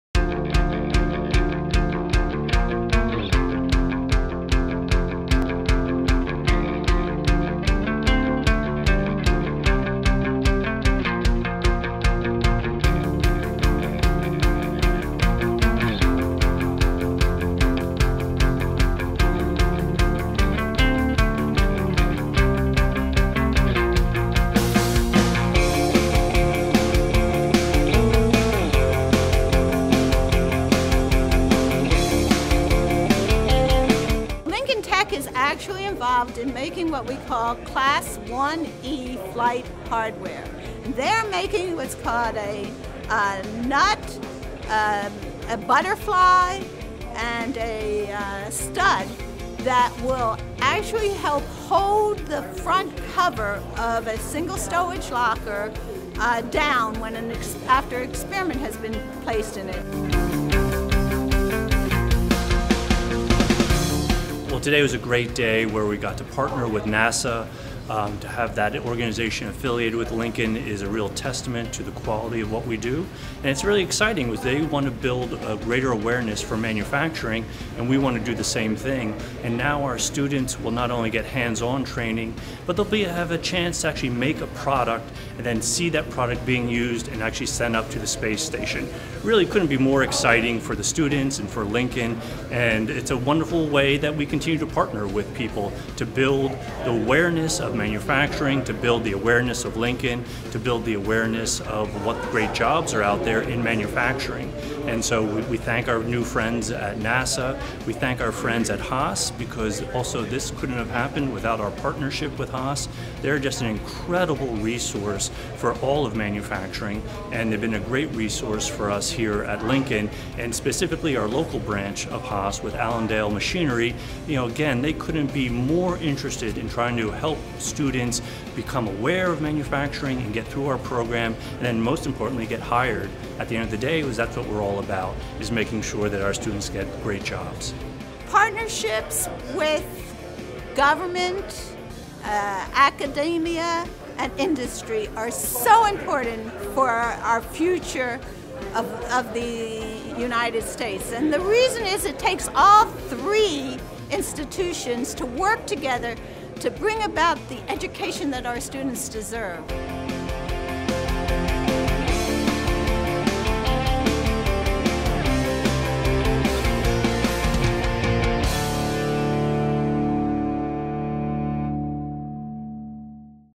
LincolnTech and NASA HUNCH Program
Students training at Lincoln Tech in Mahwah have a new out-of-this-world program option!  NASA recently selected the campus to be the newest participant in the HUNCH program: High school students United with NASA to Create Hardware.  Trainees in the CNC Machining and Manufacturing Technology program can now apply to take part in a special project building hardware components for the International Space Station (ISS).  Through the program, says Dr. Florence Gold, NASA HUNCH Project Manager, “students acquire 21st-century skills and develop a relationship with NASA that can build important professional connections.”
Using specialized Computer Numerical Control equipment donated to the school by leading manufacturer Haas Automation, Inc., Lincoln Tech students will create parts for specially-designed storage lockers aboard the ISS.  These storage lockers will hold scientific experiments conducted by NASA and other organizations from around the world. Students will build the studs, nuts and butterfly bolts that hold the lockers together, while also learning how to document their work according to NASA specifications.
Launch your own exciting new career, with training from Lincoln Tech!